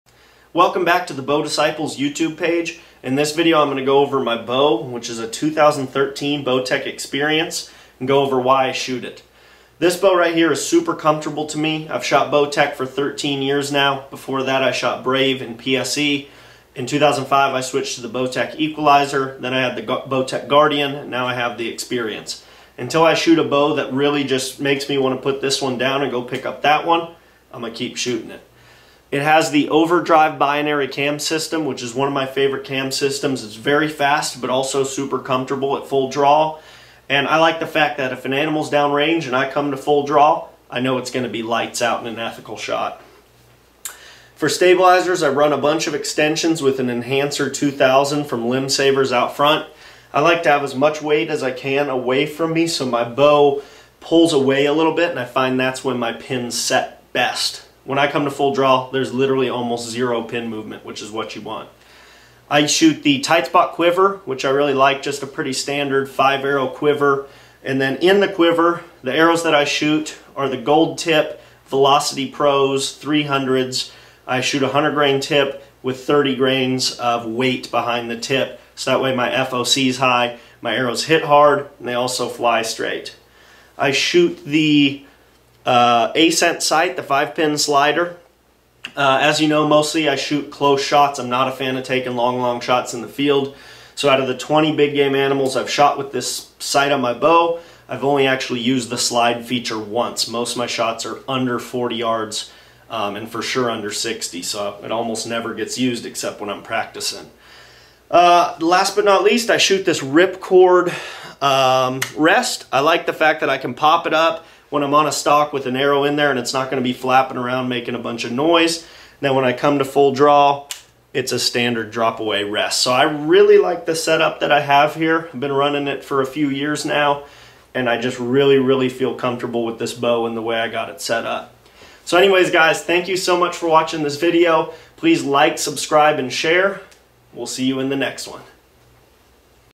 BOW CHECK: 2013 Bowtech Experience
In this video, James walks us through the set up on his Bowtech Experience.